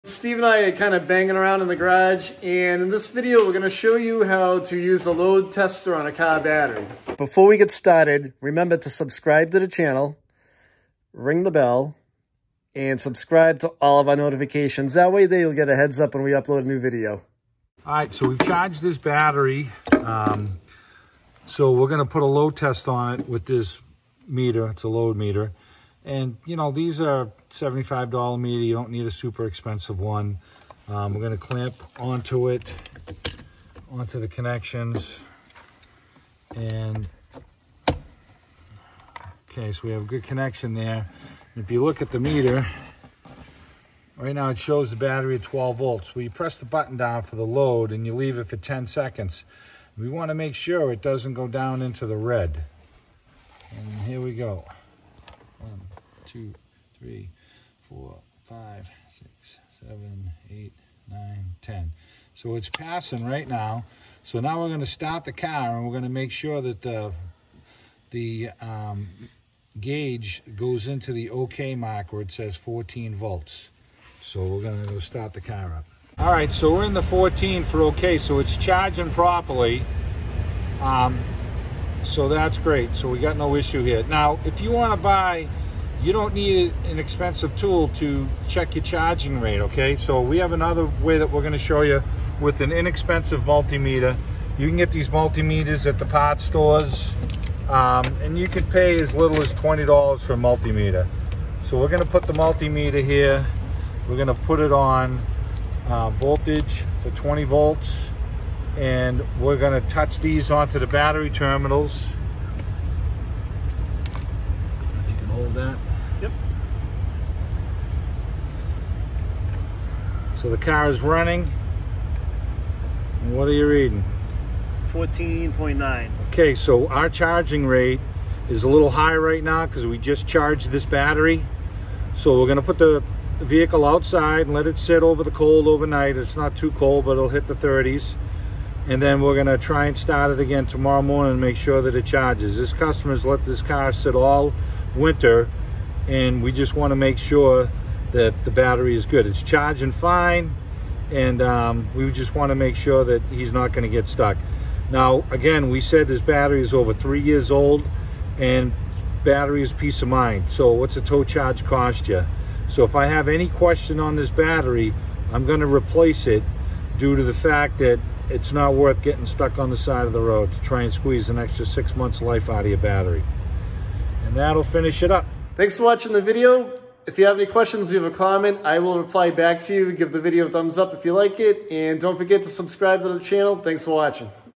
How To Use A Battery Load Tester
This video will show you how to use a battery load test tool. This tool will help you to diagnose a potentially bad battery and also how well the battery is being charged.